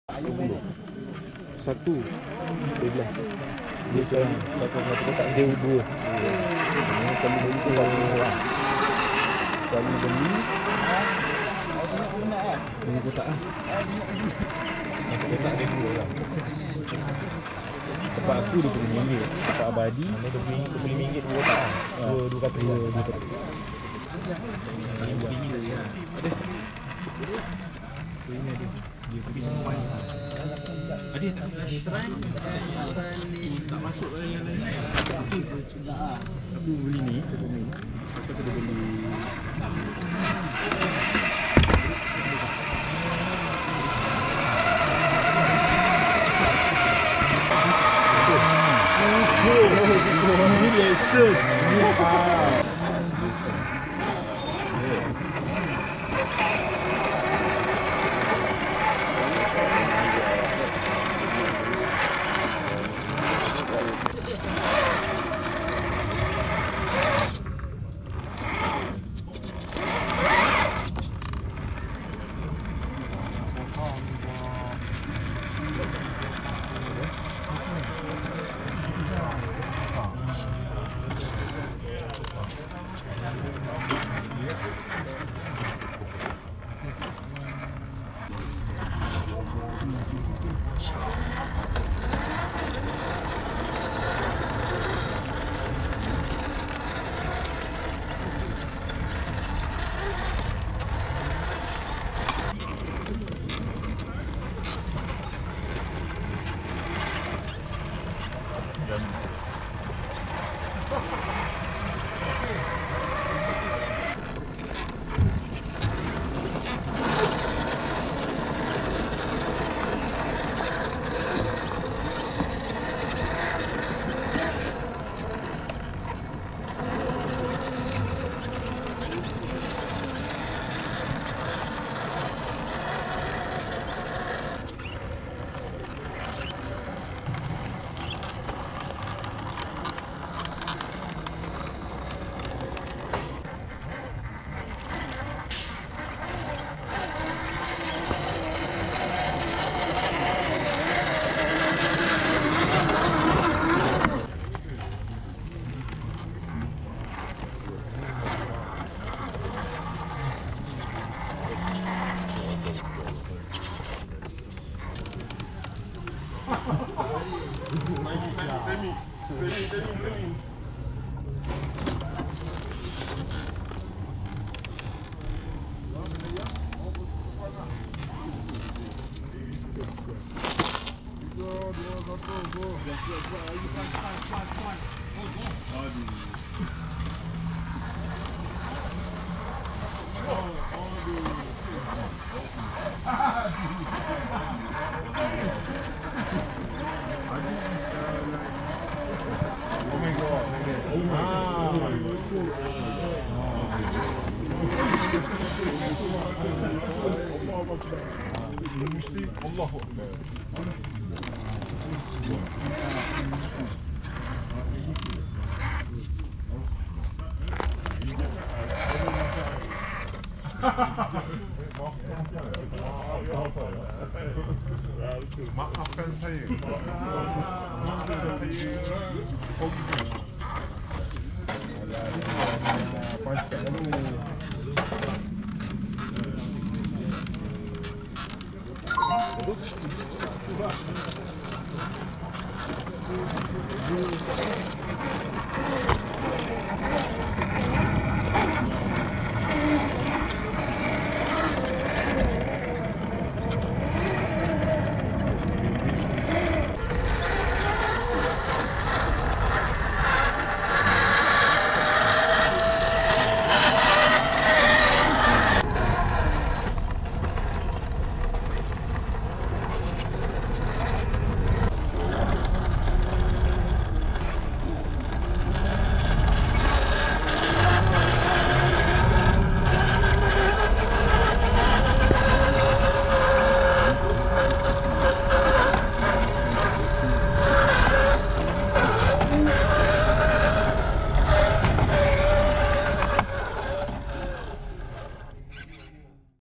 RC 1/10 Scale | BRX02 Land Rover D110 Camel Trophy | Desaminium Track | 03092023
Me and my Boom Racing BRX02 Land Rover D110 Camel Trophy crawling around Desaminium Track after event competition CAMEL TROPHY BORNEO TRIBUTE.